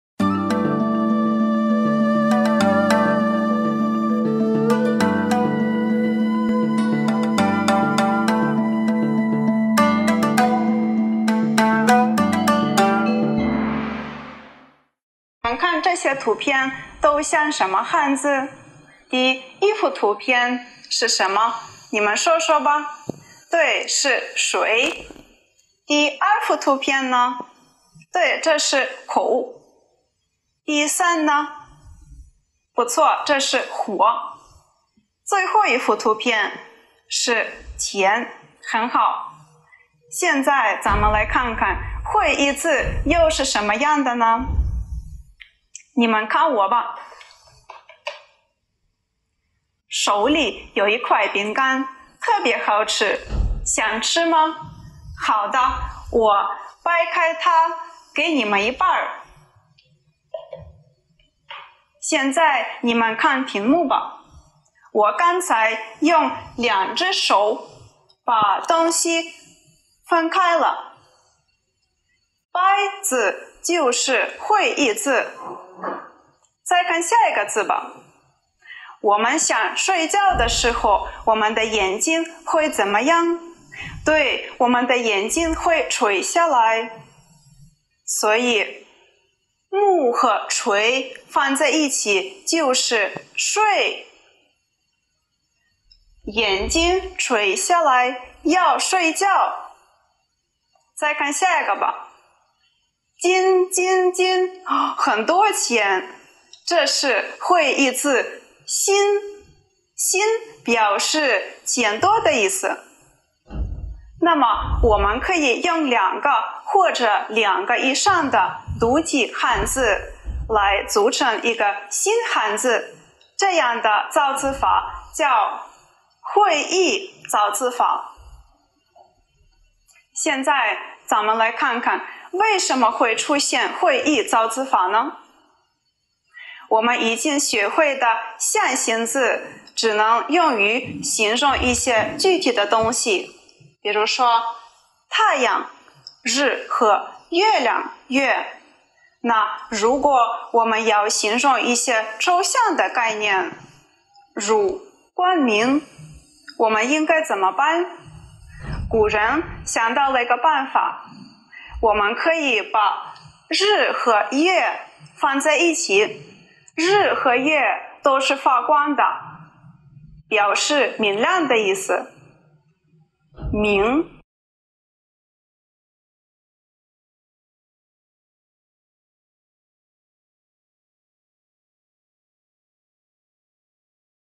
喀秋莎老师教会意字
会意字，是指用两个及两个以上的独体汉字，根据各自的含义所组合成的一个新汉字，这种造字法就叫作会意，属于六书中的一种。用会意造字法造出的汉字就是会意字。
喀秋莎老师从象形造字法的局限性谈起，通过简单生动的字例说明会意造字法的优越性，让学生在识字学习中感悟汉字的魅力。